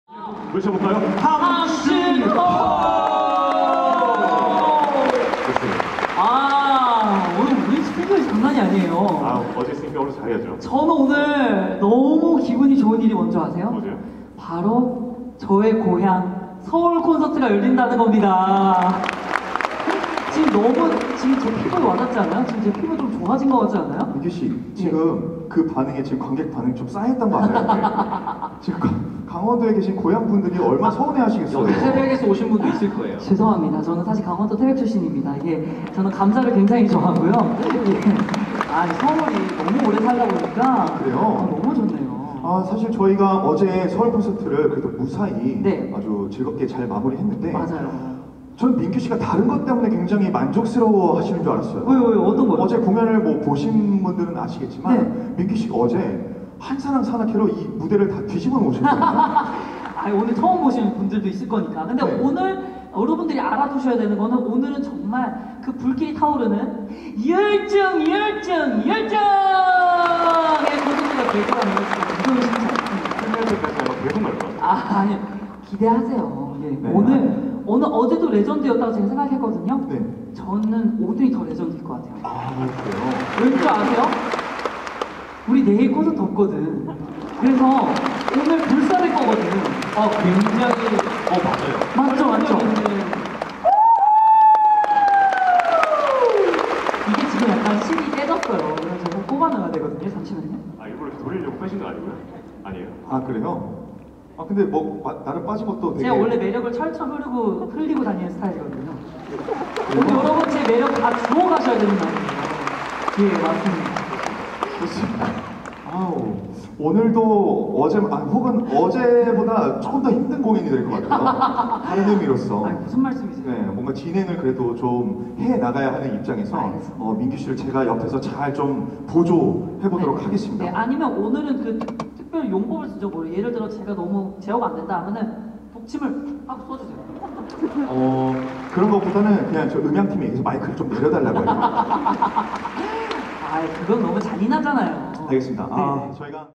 [ 210627 | The Forestella in 서울 ] 토크험난주의보 F. 배두훈
210627 | The Forestella in 서울
토크험난주의보
F.배두훈

::: 2차 가공 금지
::: 지진주의
::: 작게보세요
::: 흔들림주의
::: 저퀄주의
::: 포커스나감주의
::: 잘림주의
::: 초반어지러움주의